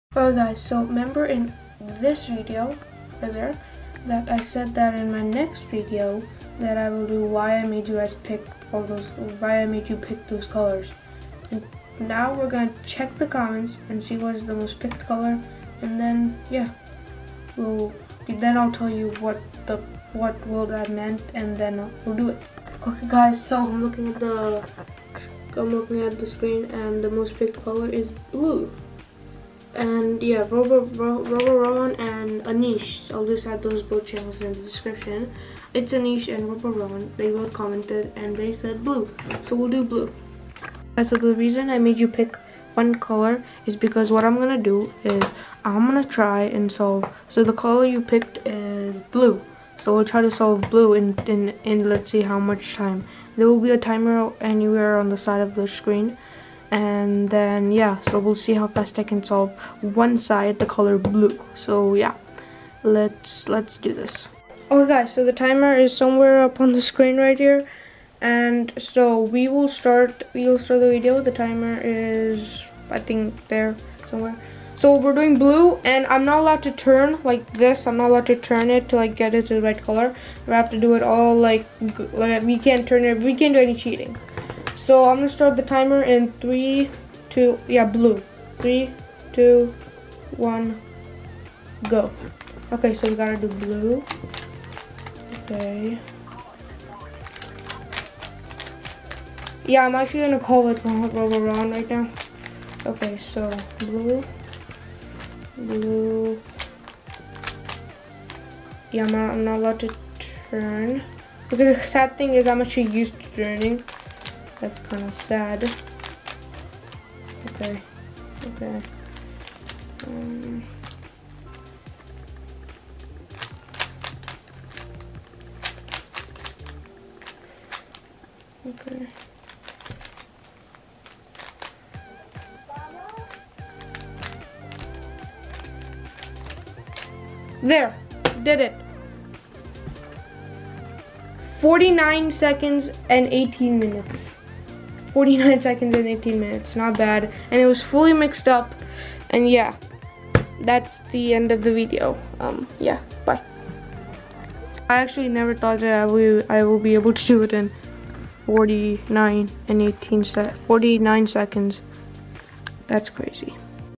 I fast can solve one side of a rubix cube
I can't believe I did that
it was actually 46 seconds not 49